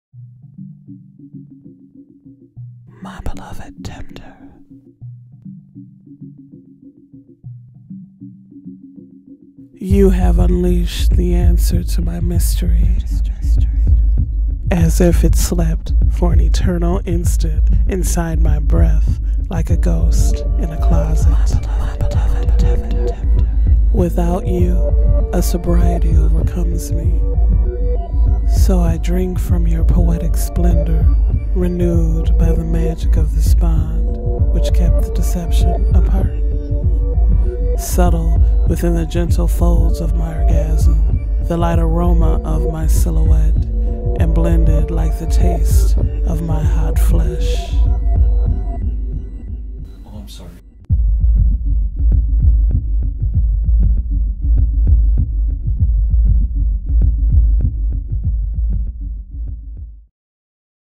My Beloved Tempter: A Valentine Tribute
If your speakers cannot take the bass, then headphones sound best!

My Beloved Tempter

You have unleashed the answer to my mystery
As if it slept for an eternal instant inside my
breath like a ghost in a closet

Without you a sobriety overcomes me
So I drink from your poetic splendor
renewed by the magic of this bond
which kept the deception apart

Subtle within the gentle folds of my orgasm
the light aroma of my silhouette
and blended like the taste of my hot flesh